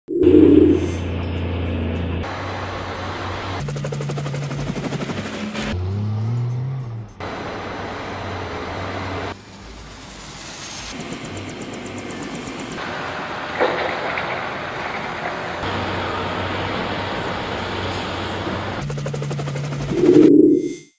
Megaoperativo en Irapuato vs. huachicol
Seguridad🚨 | Autoridades montan megaoperativo en Irapuato; van por huachicoleros y clausuran 4 tomas clandestinas.